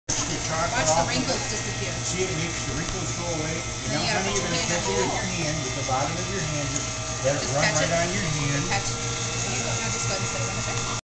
mary's bday
making candy at the sweet shoppe